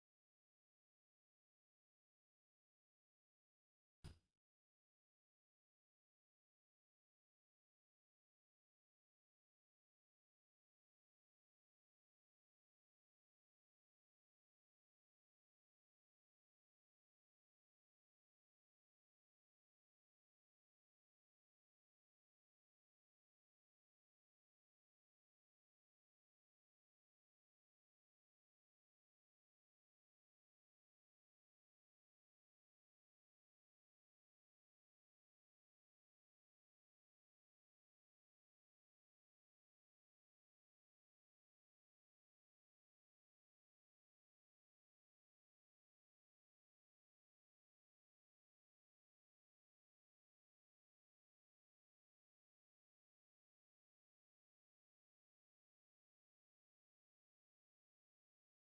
Thermal Volumetric Expansion of Metals
This experiment demonstrates the increase of volume of a metal ball when it is heated.